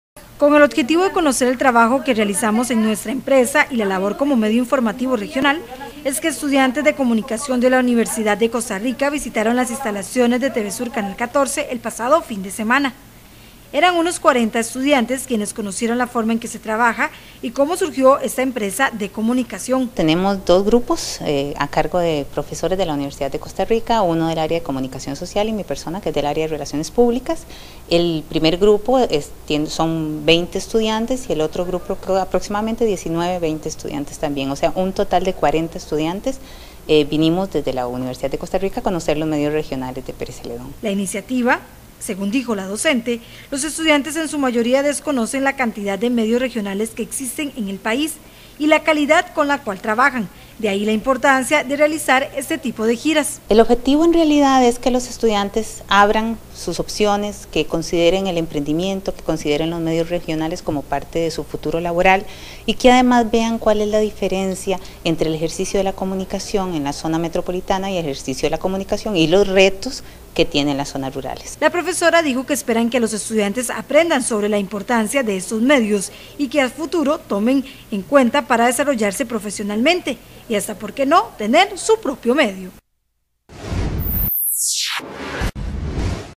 Estudiantes de Comunicación de la UCR aprenden sobre la labor de Tv Sur Canal 14
Con el objetivo de conocer el trabajo que realizamos en nuestra empresa y la labor como medio informativo regional, es que estudiante de Comunicación de la Universidad de Costa Rica, visitaron las instalaciones de Tv Sur Canal 14 el pasado fin de semana.